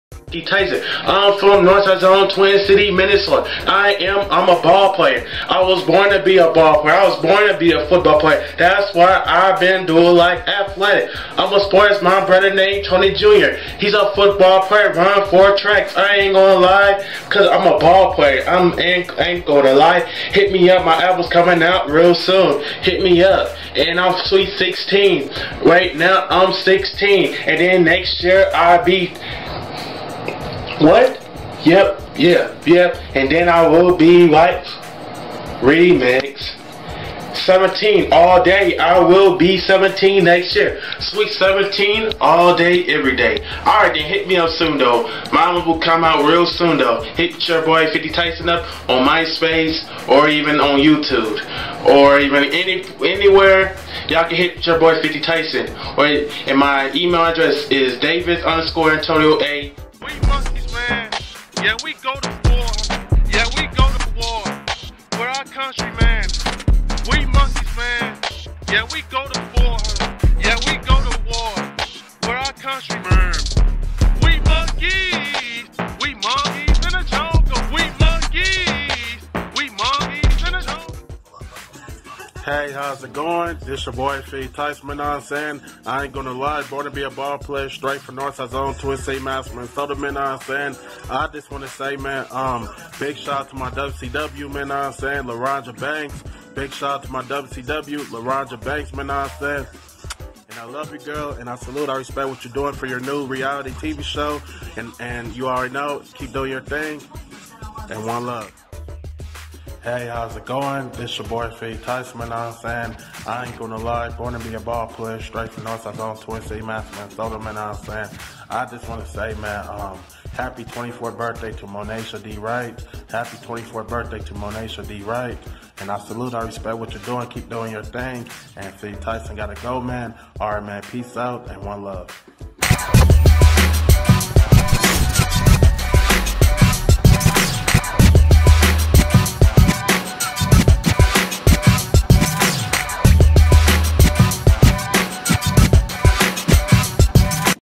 50 Tyson: Ever Wonder What Happened to Him???
Update on how internet sensation 50 Tyson is doing in 2020.
Video begins with a flashback of what made him famous a decade ago.